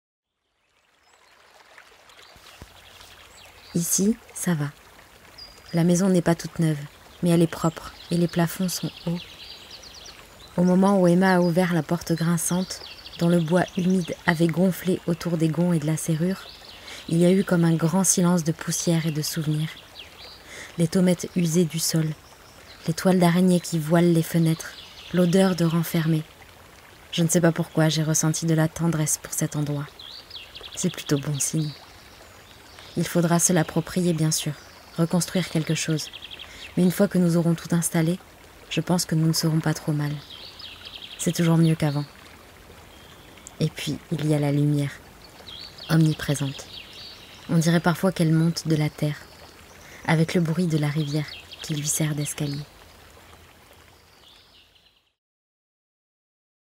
25 - Ici, ça va - Thomas Vinau
"Ici ça va. La maison n'est pas toute neuve mais elle est propre et les plafonds sont hauts. Au moment où Ema a ouvert la porte grinçante, dont le bois humide avait gonflé autour des gonds et de la serrure, il y a eu comme un grand silence de poussière et de souvenirs. Les tomettes usées du sol, les toiles d'araignées qui voilent les fenêtres, l'odeur de renfermé, je ne sais pas pourquoi j'ai ressenti de la tendresse pour cet endroit. C'est plutôt bon signe. Il faudra se l'approprier bien sûr, reconstruire quelque chose, mais une fois que nous aurons tout installé, je pense que nous ne serons pas trop mal. C'est toujours mieux qu'avant. Et puis il y a la lumière. Omniprésente. On dirait parfois qu'elle monte de la terre. Avec le bruit de la rivière. Qui lui sert d'escalier."
Extrait de "Ici ça va", de Thomas Vinau, aux @editions1018
J'ai pour l'occasion emprunté les escaliers du si beau cinéma qu'est @cinemautopiabordeaux
J'allais voir Annie Colère ce jour là (superbe 💙), et je n'ai pas résisté aux rayons de lumière dans l'escalier ❤️
Je vous envoie des éclats de soleil à toustes